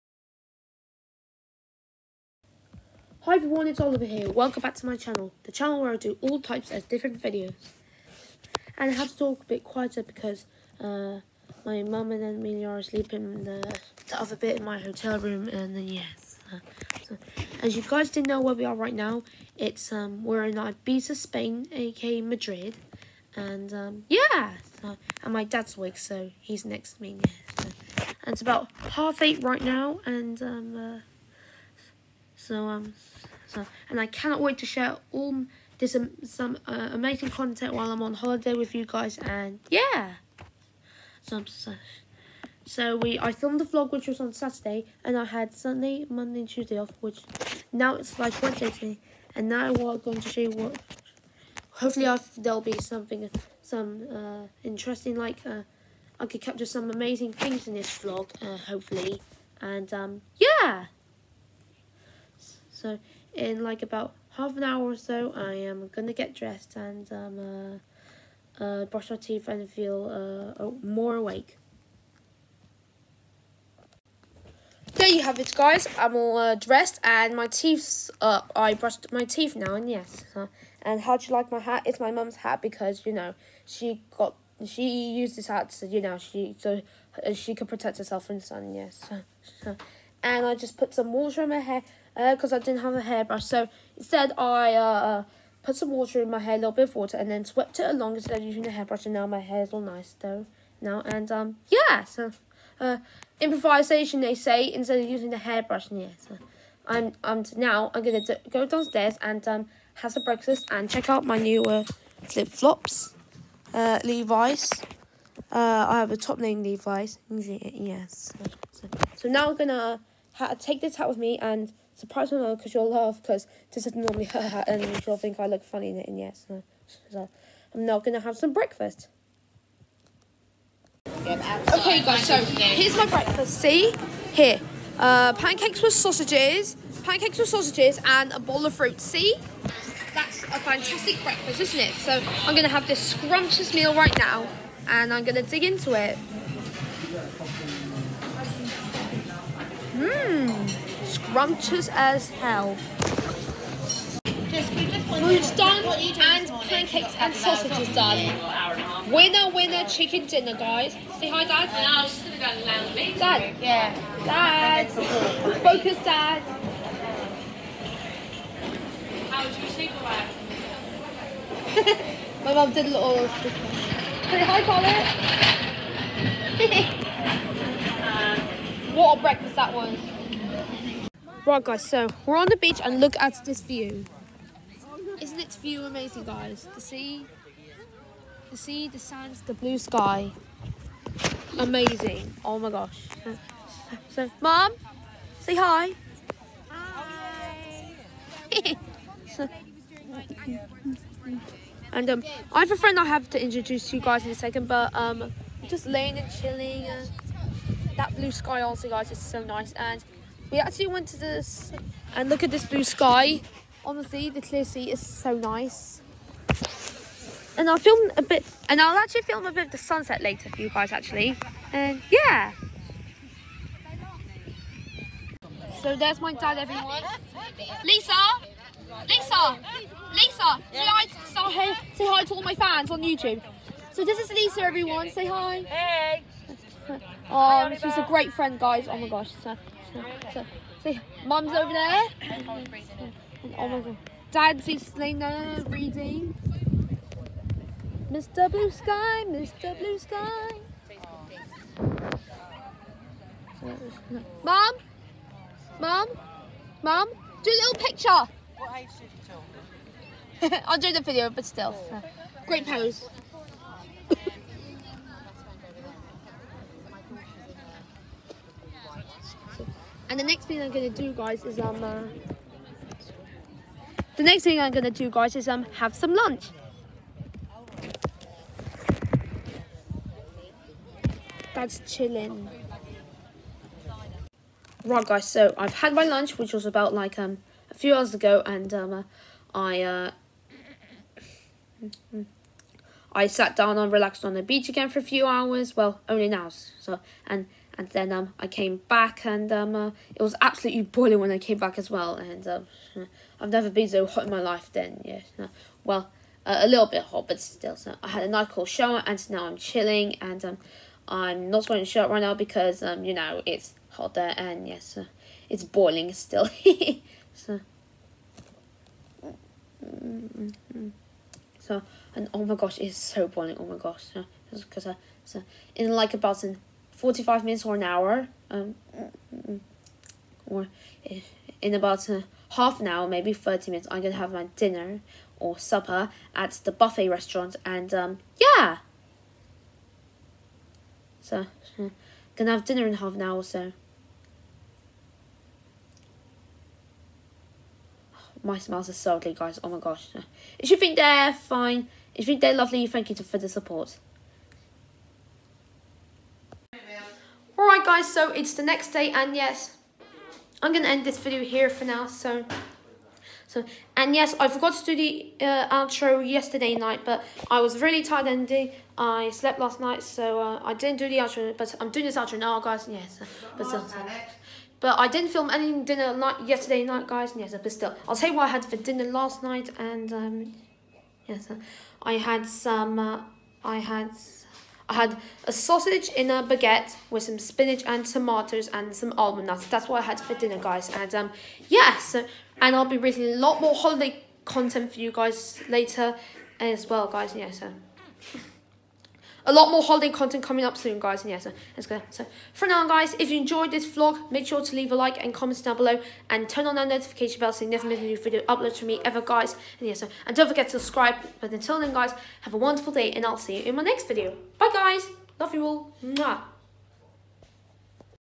Just a normal day in Ibiza. HOLIDAY VIBESSSSSS!!!!!!!
We had another great day here in Ibiza Spain. Sorry if it took so long to upload. Hope you enjoy the vlog. Love Oliver. Love you all SOOOOOOOO MUCH!